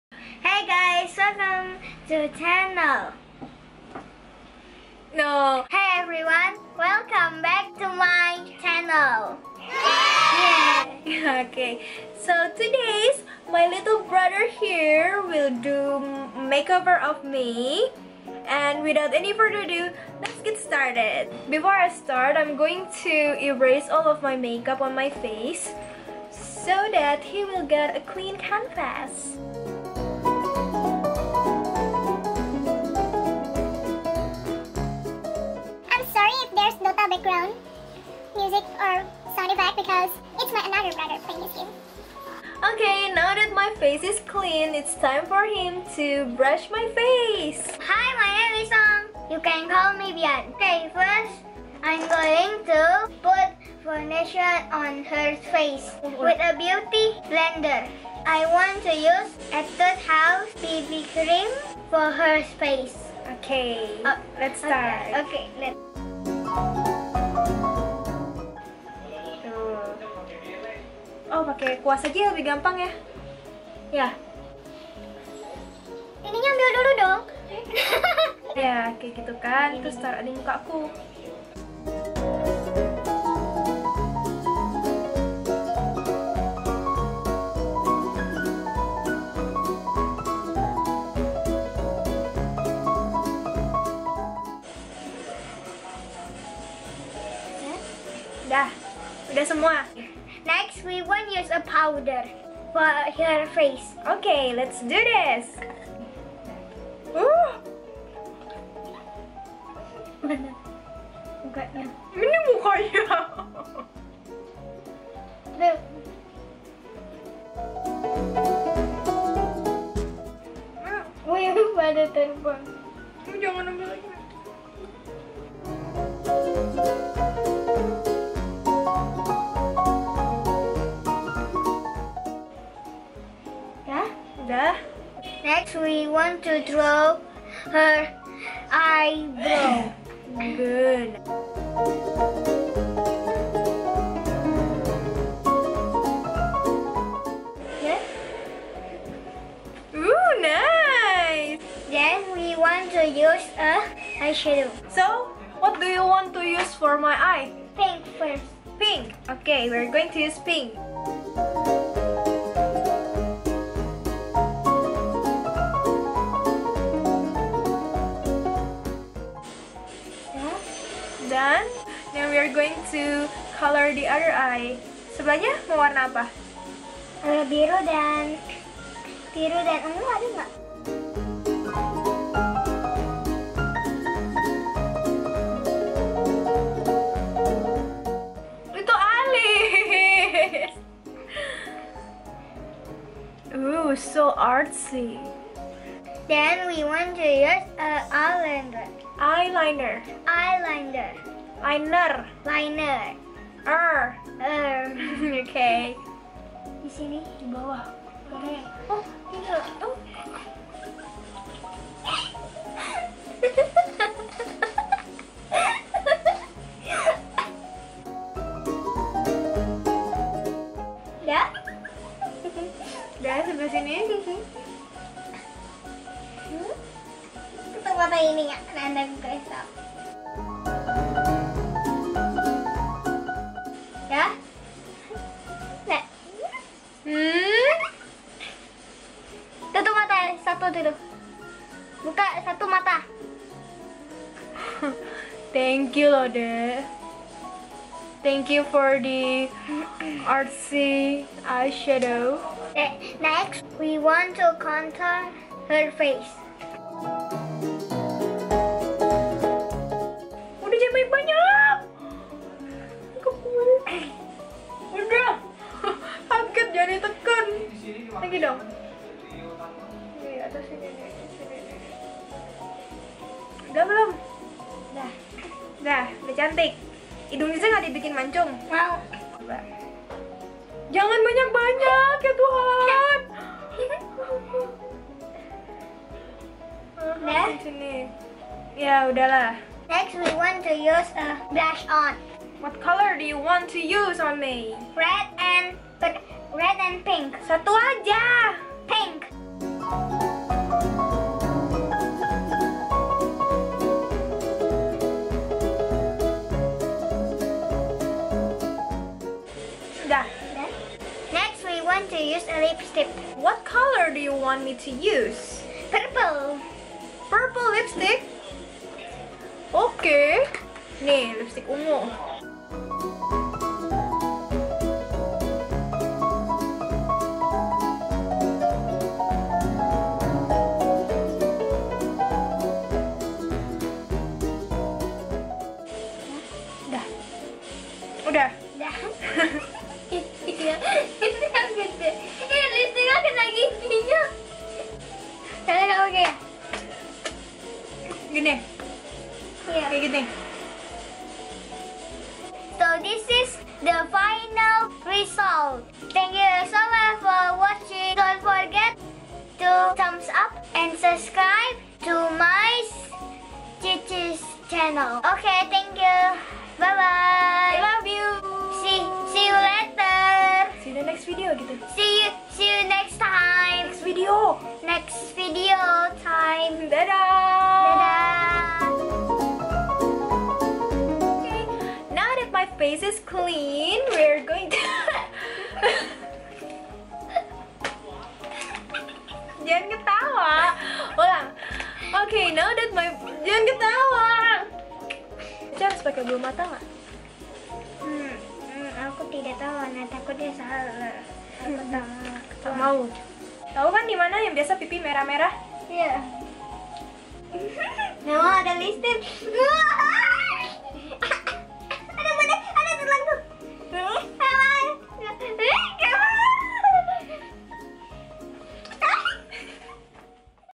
LIL BRO DO MY MAKEUP
Hey peeps! welcome back to my channel! today's video i'm featuring my cute little brother Ong Fabian Antonius :) he's 8 years old. aku tau dia ngomongnya masih terbata-bata karena dia telat belajar ngomongnya, jadi tolong dimaklumin yah :)